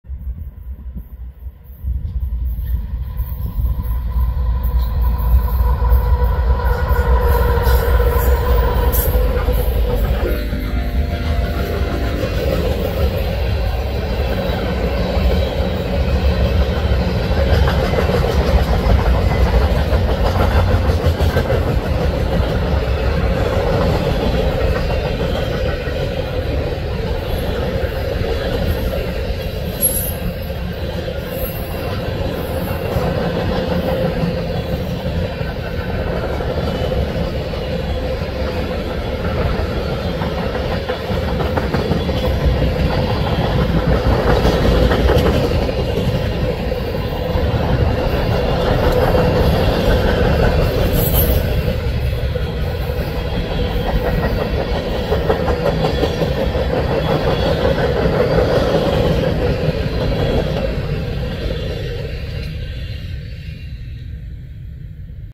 Canadian National - Eastbound Freight 16 Oct 24
Potterville, MI  -  Lake Alliance Park at 1000 EDT

CN ES44AC 2880 and 3947 (Ex-CREX 1312)